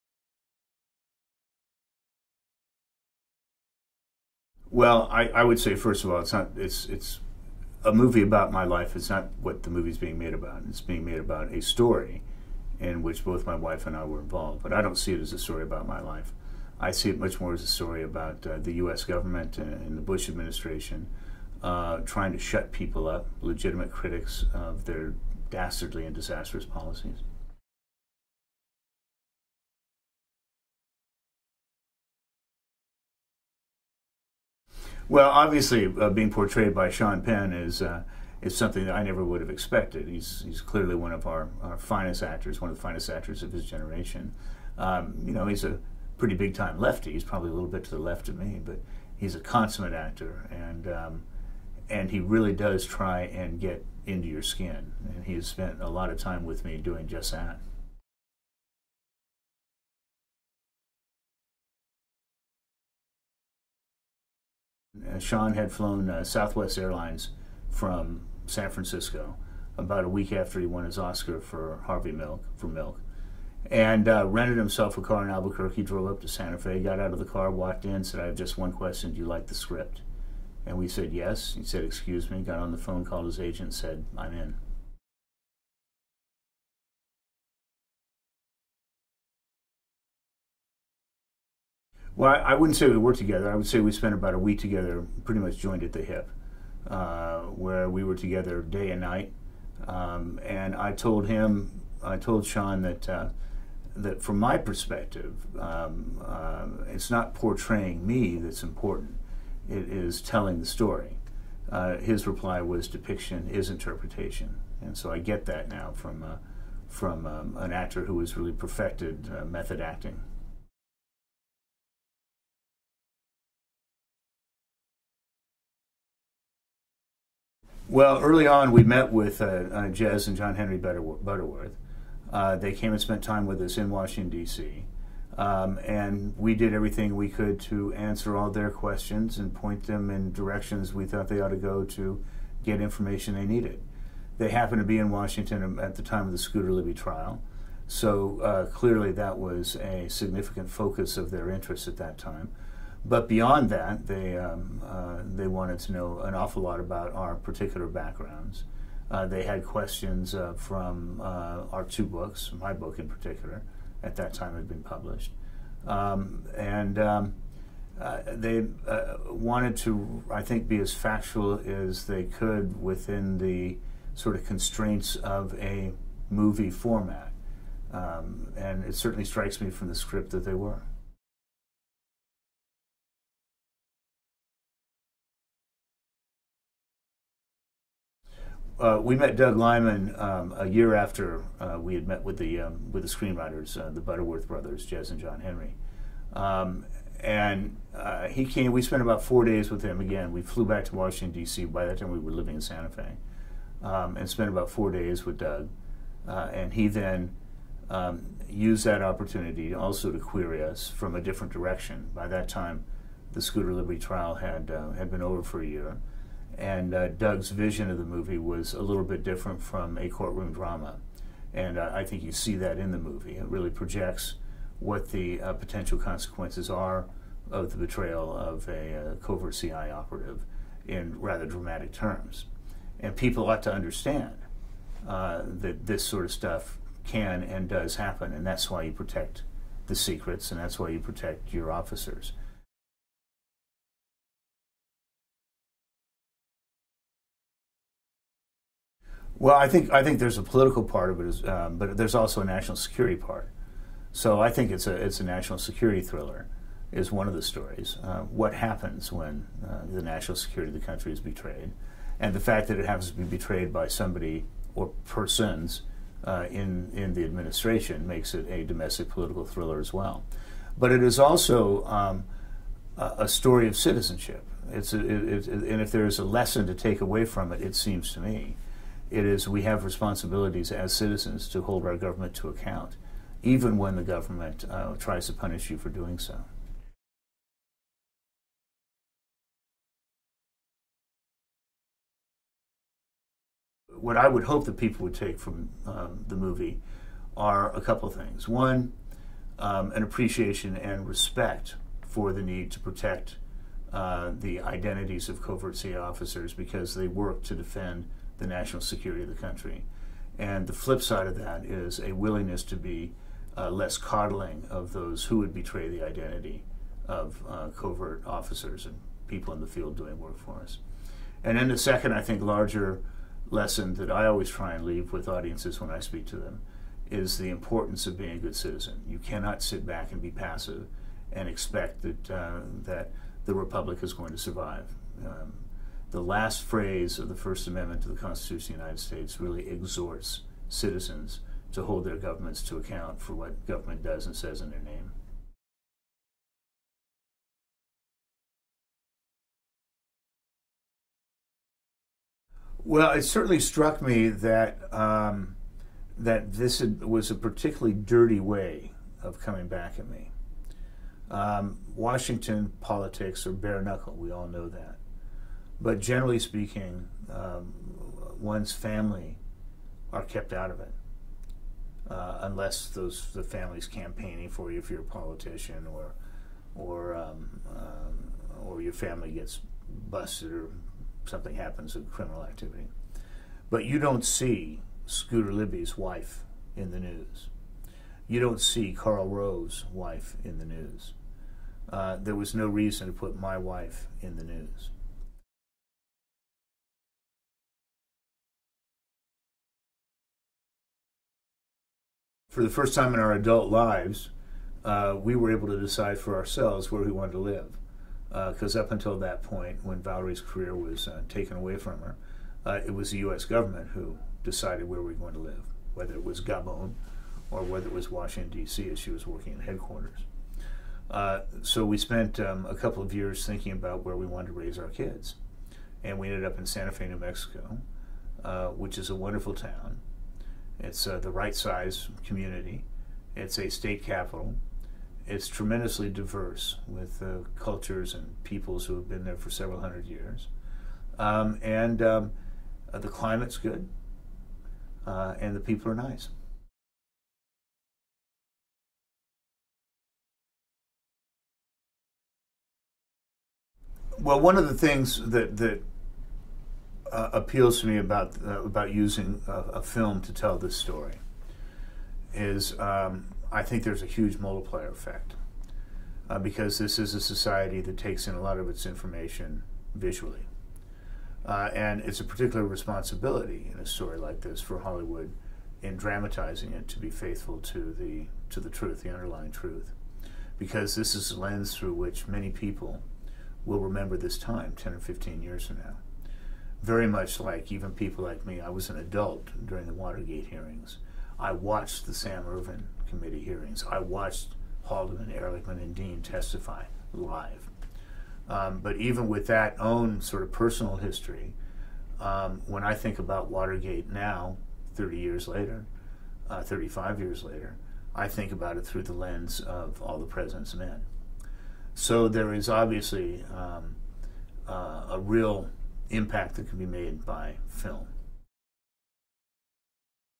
Fair Game: Interview - Joe Wilson
Interview with the real Joe Wilson on Fair Game.